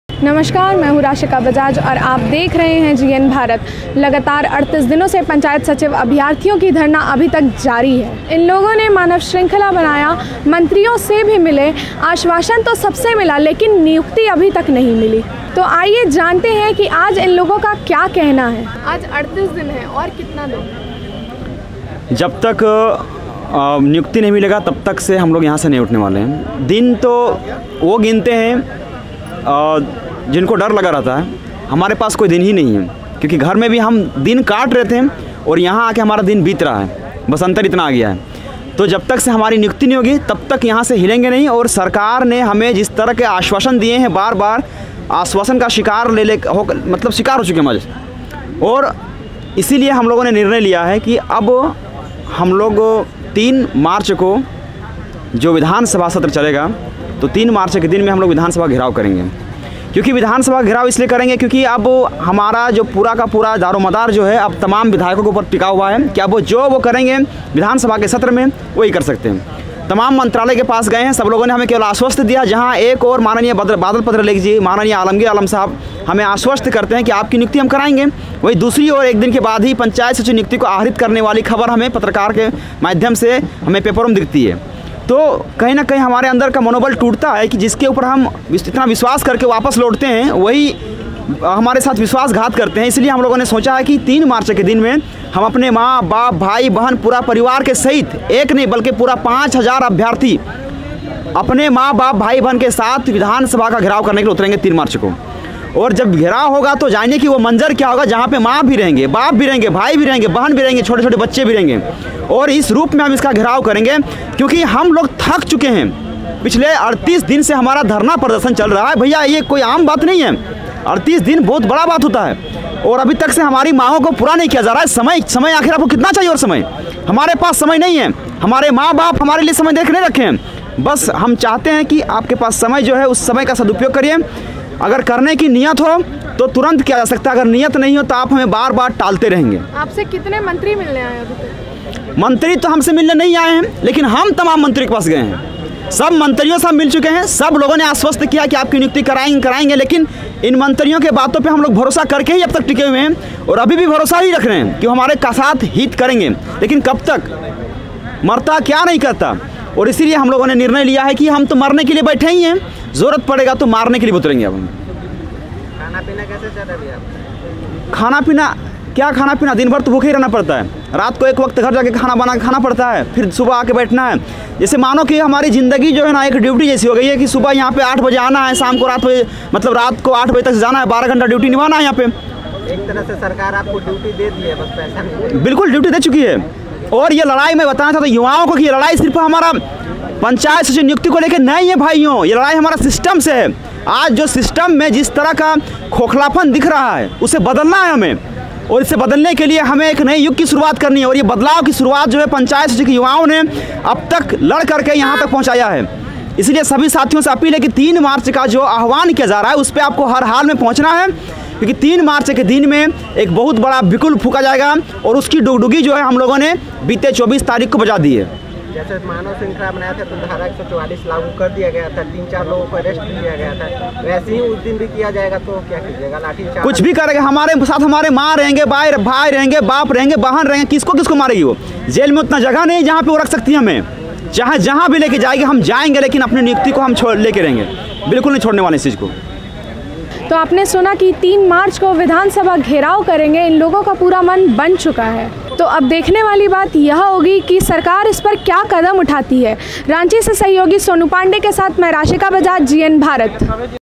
3 मार्च को विधानसभा घेरेंगे : पंचायत सचिव अभ्यार्थी || Panchayat Sachiv Abhyarthi || Ranchi News
3 मार्च को विधानसभा घेरेंगे : पंचायत सचिव अभ्यार्थी